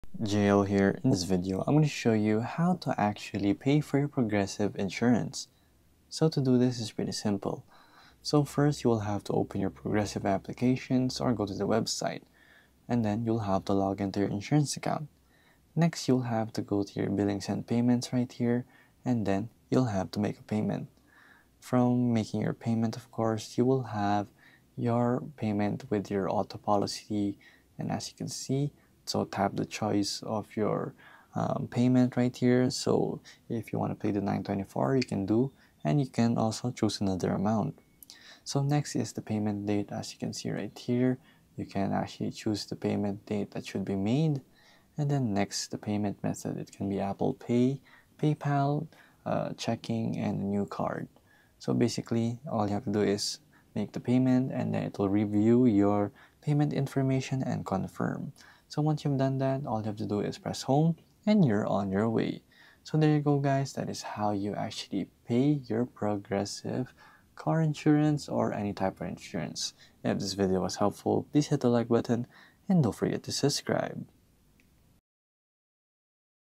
How To Pay Progressive Insurance | Step By Step | 2025
👉  In this tutorial, we will show you how to easily pay your Progressive insurance bill online. Follow along as we guide you through the simple steps to make a payment and keep your coverage up to date.